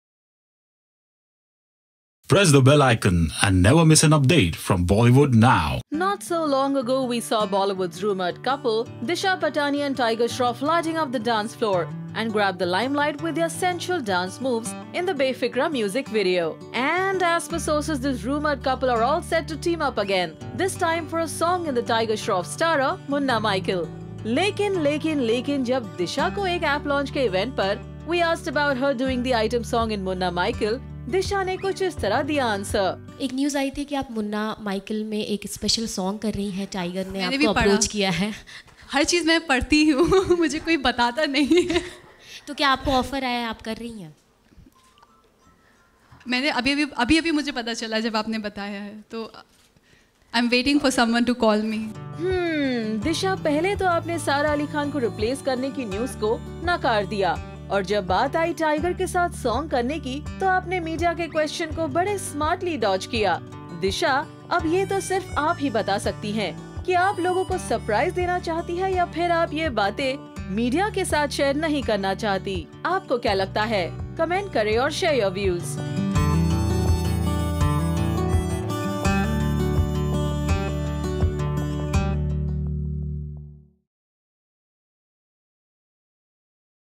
Disha Patani And Tiger Shroff Are BACK TOGETHER
Disha Patani denies working with Tiger Shroff again and clears all the rumours about her item song in Munna Michael, to know more about it check it out here.